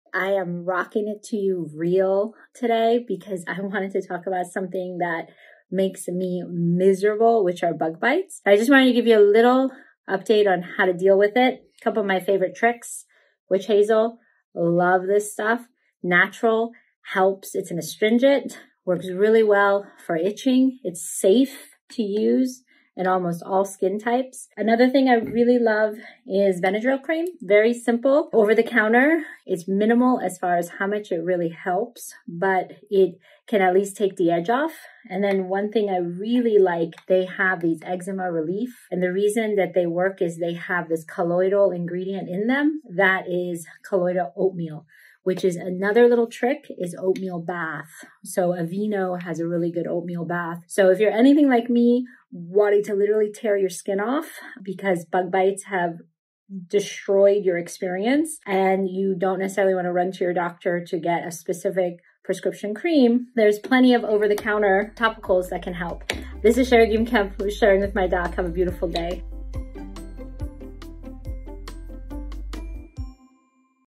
Medicine Cabinet Must-Haves for Bug Bites & Poison Ivy | Sharing With My Doc with Dr. Sharagim Kemp
Run, don't walk, to your nearest drug store and pick up these four over-the-counter topicals to keep in your medicine cabinet for things like bug bites and poison ivy! You'll be glad you have them on hand if (when!) you need them!
𝐑𝐞𝐬𝐨𝐮𝐫𝐜𝐞𝐬
· T.N. Dickinson's | Witch Hazel All Natural Astringent
· @BENADRYL Itch Stopping Cream Extra Strength Itchy Skin and Rash Relief with Diphenhydramine